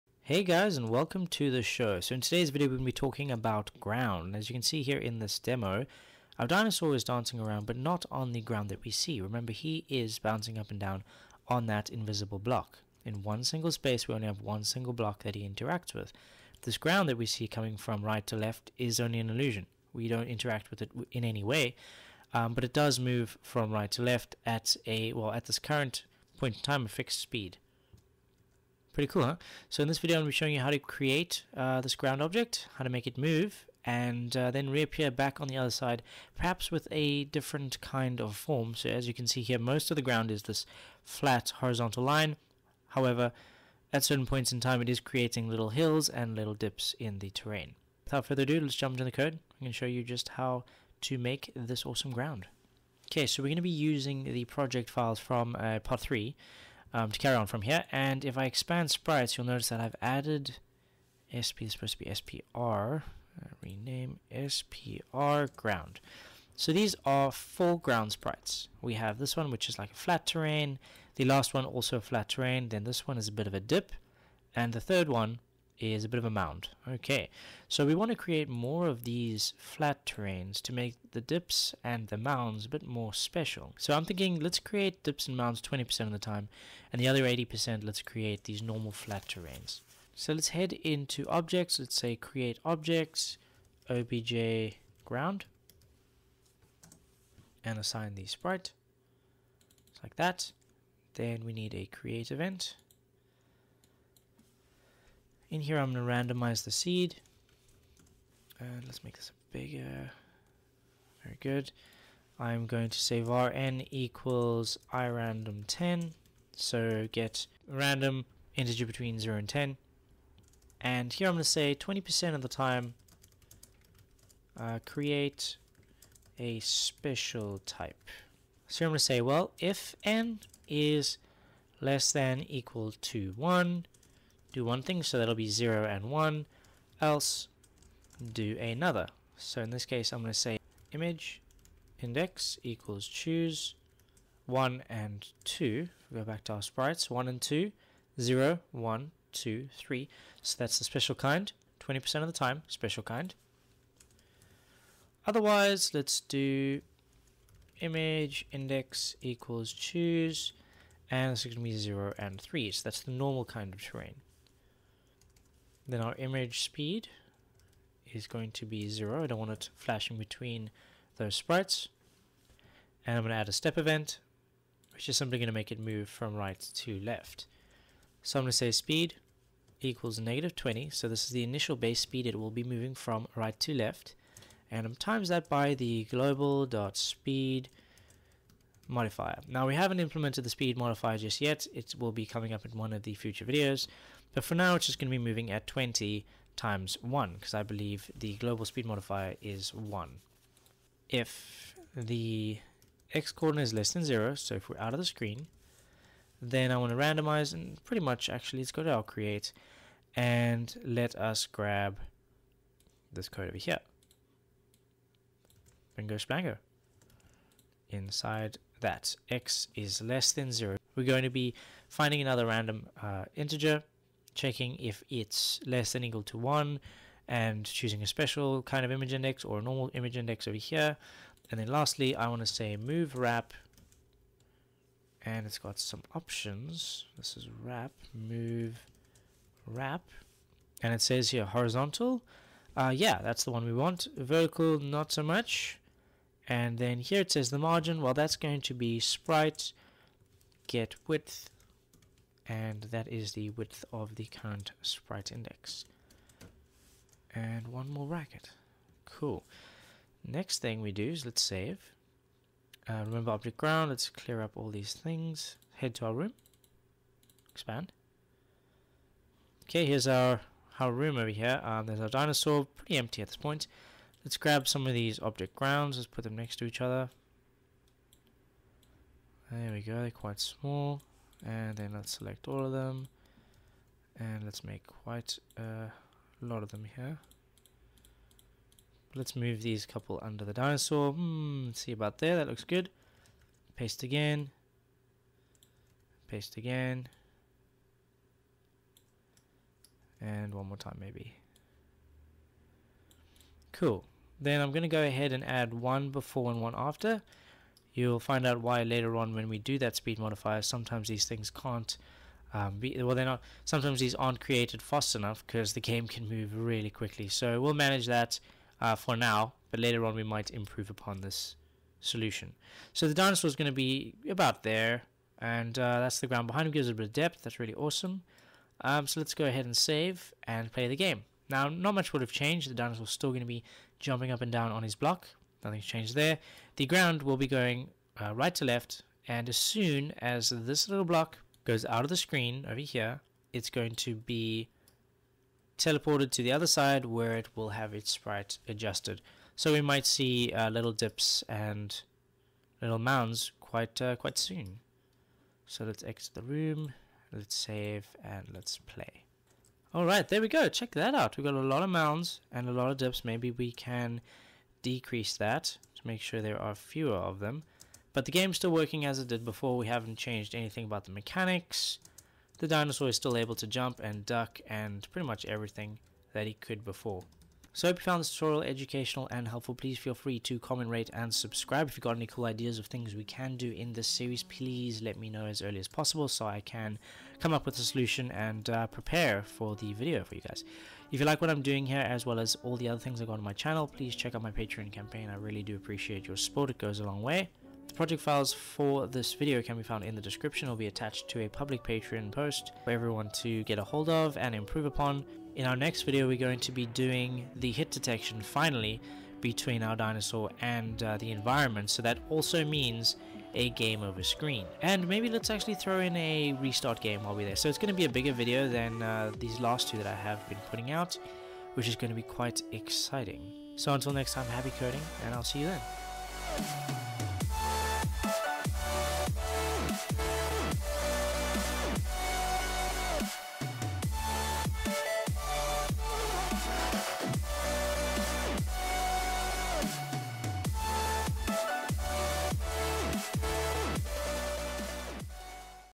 Make an Endless Runner Game - Adding the Ground - GameMaker Studio 2
Have you ever wondered what it takes to recreate Google Chrome's Offline T-Rex Run game? Well, now you can follow me as we recreate it together! We'll be using GameMaker Studio 2.

DOGE ▶ DAVYUBtcseqtZaEuzQmCnUXntJmizbbd7x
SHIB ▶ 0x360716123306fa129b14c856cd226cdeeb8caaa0